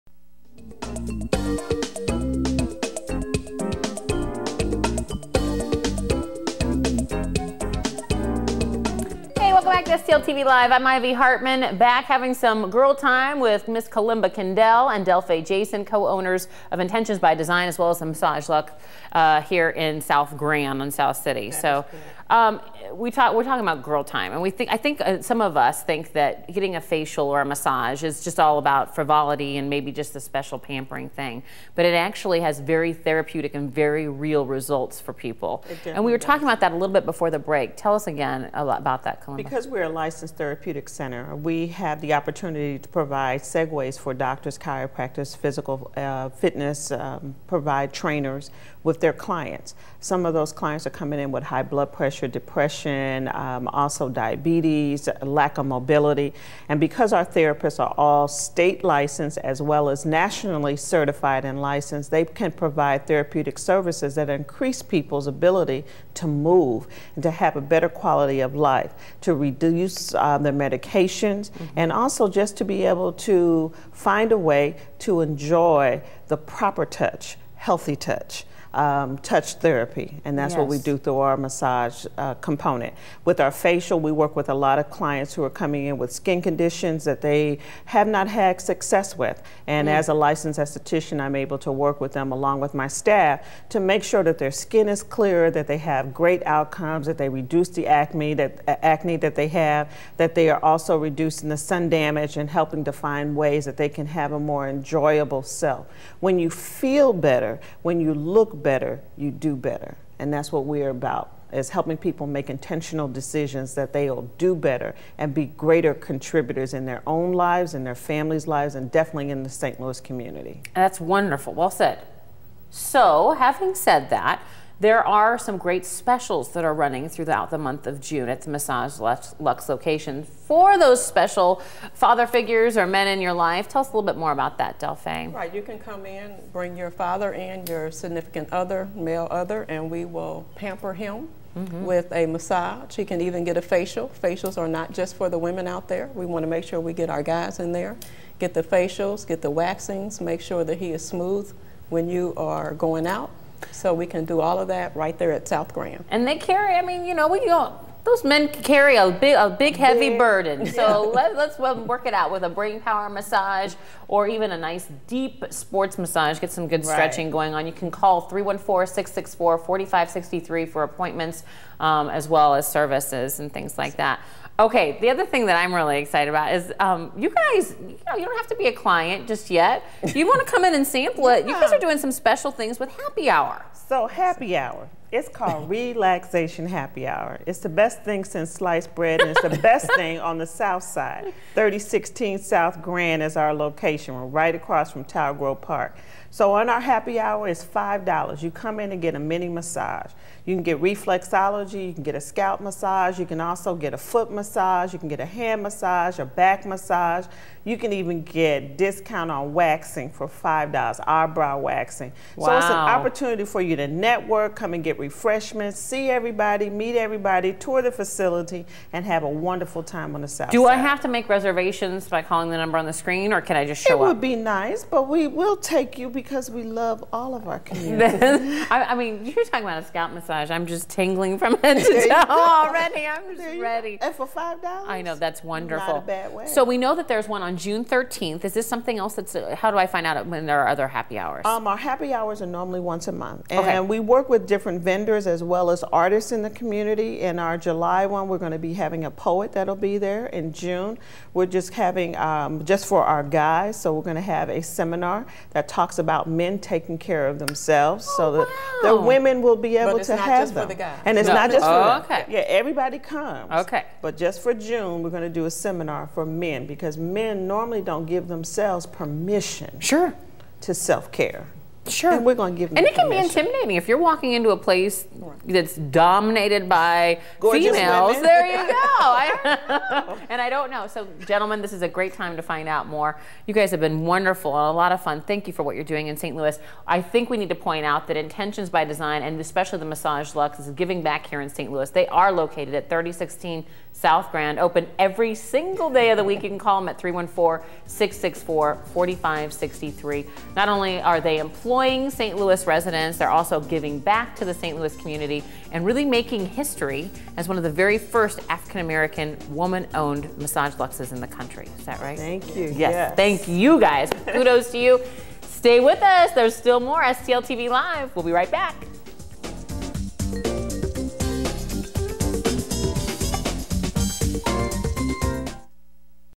STL TV LIVE -- Intentions By Design (part 2 of 2)
Host: Ivy Hartman
Guests: K. Kalimba Kindell / Owner/Licensed Esthetican, Massage LuXe South Grand & Delfaye Jason / Co-owner, Massage LuXe South Grand

 June 2013 Specials / Brain Power Massage (30 minutes -- scalp, neck, shoulder, and face) / Sports Massage (60 minutes -- deep tissue with stretching) / Relaxation Happy Hour on June 13 5:30pm (mini massages, tours, service/product discounts, seminar, refreshments)